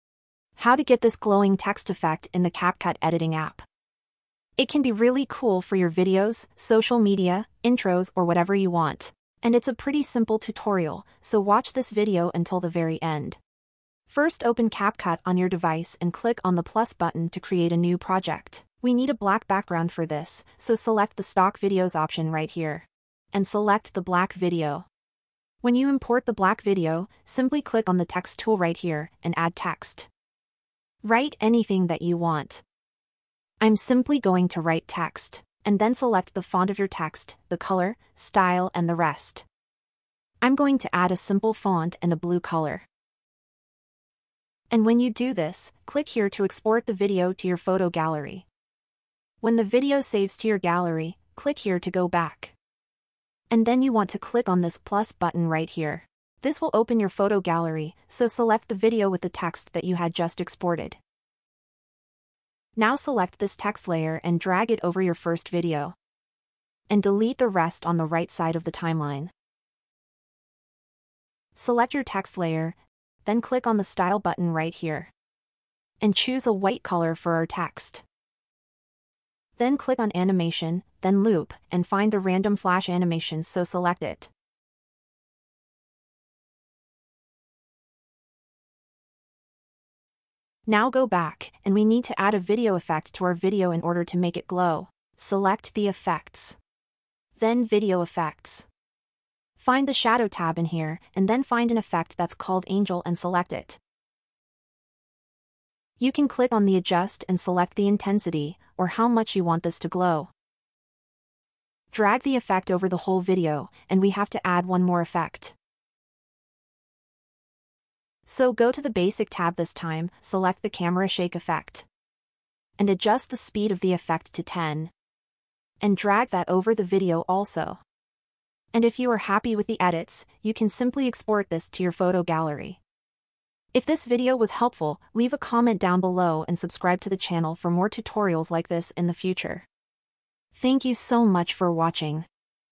How To Get Glowing Text In CapCut | CapCut Glowing Text Tutorial 2023
Let’s learn how to get the glowing text in CapCut. It’s a really cool effect, and it’s really simple to get it.